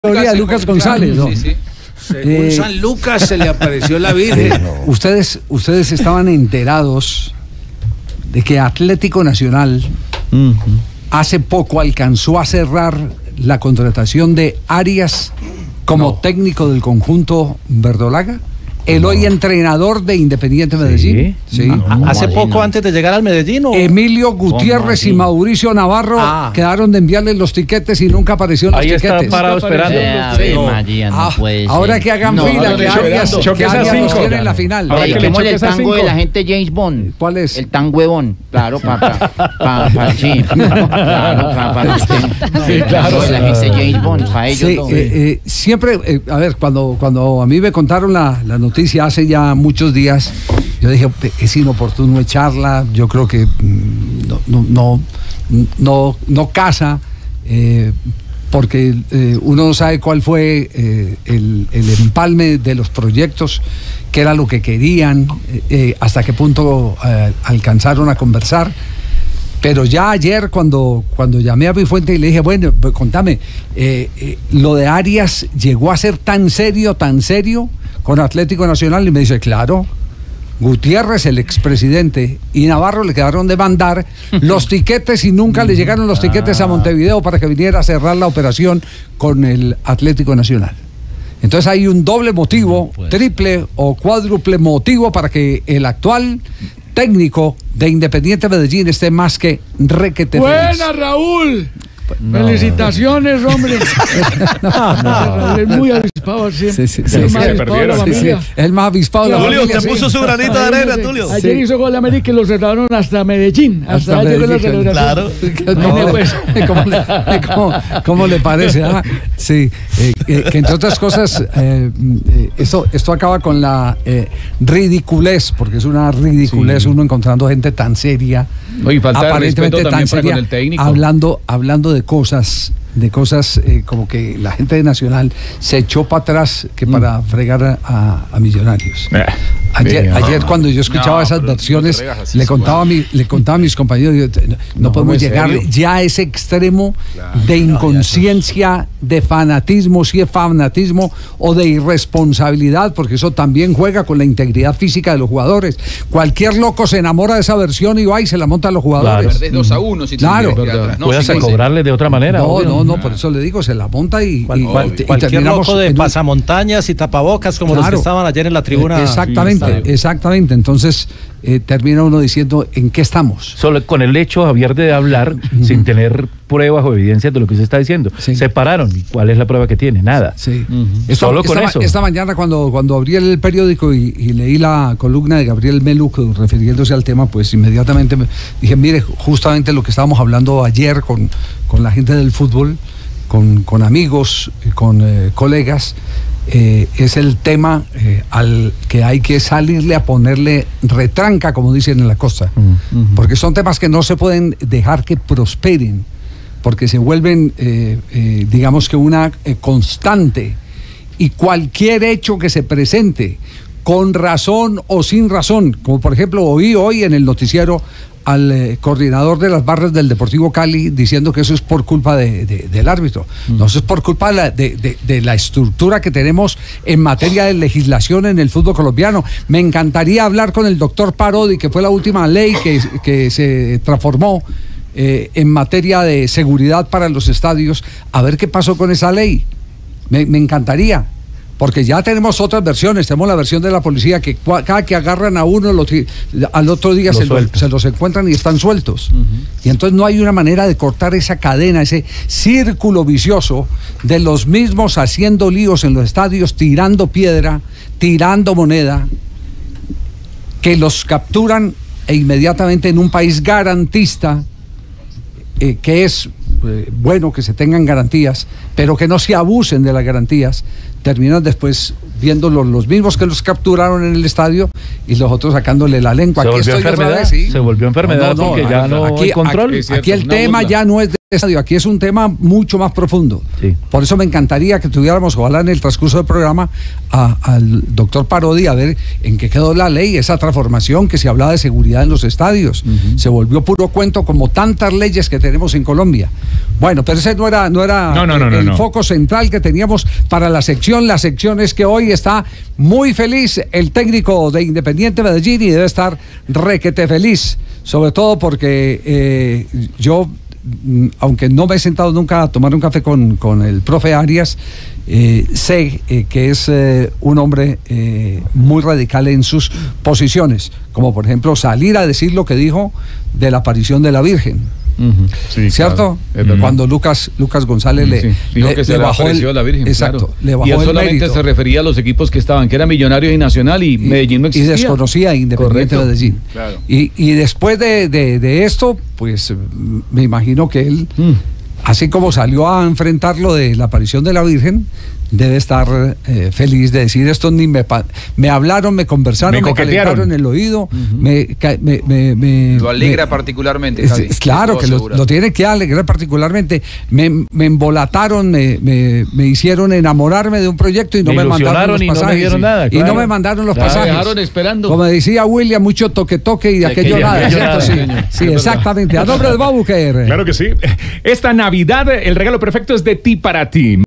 Nacional intentó contratar a Alfredo Arias; el técnico se quedó esperando tiquetes: Blog Deportivo
El técnico uruguayo Alfredo Arias ha ganado los cuatro clásicos más recientes, dos de ellos por los cuadrangulares que aún se disputan en la Liga Betplay.